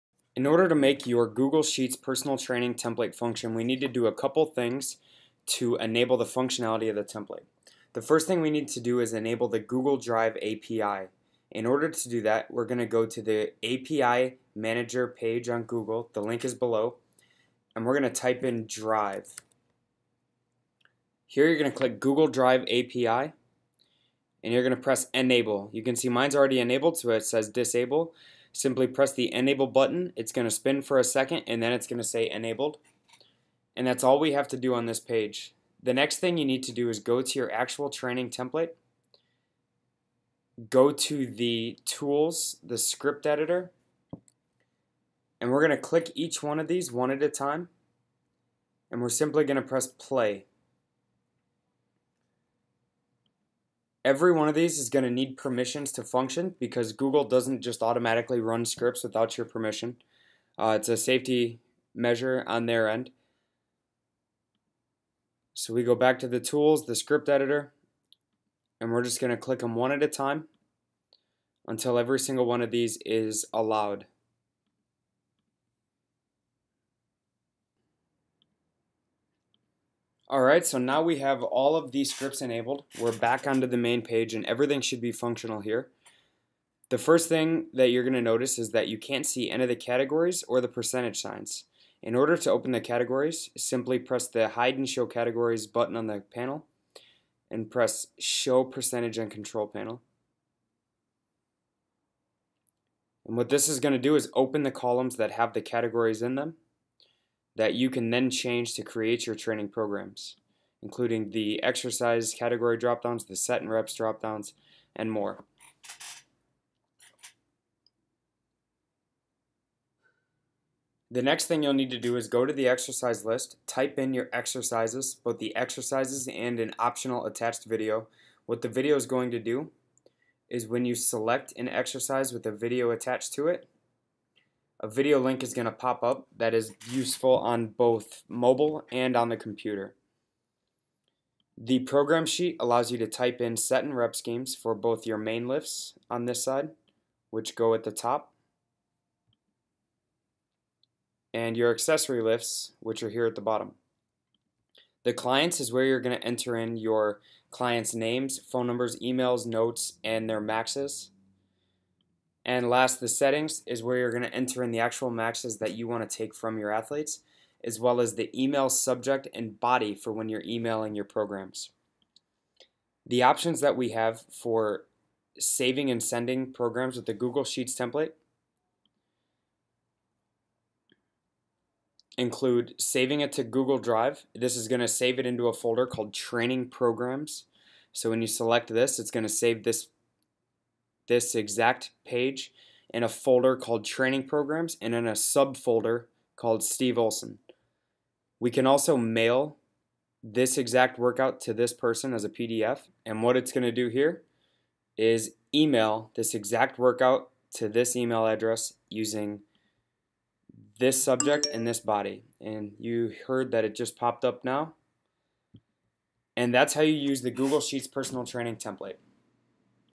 Setting up your google sheets personal training template
How to set up your brand new google sheets personal training template from excel training designs!